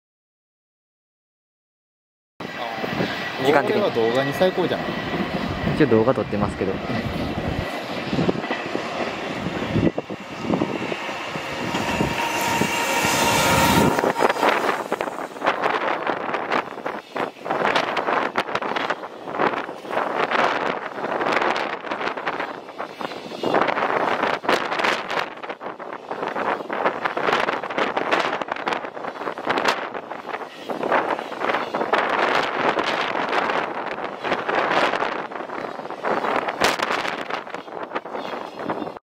大雪の5085レ 能登川駅 通過シーン
12月26日撮影　能登川駅にて　5085レ　EF210-6＋EF210−？
周りの方の音声が入っていますご了承下さい。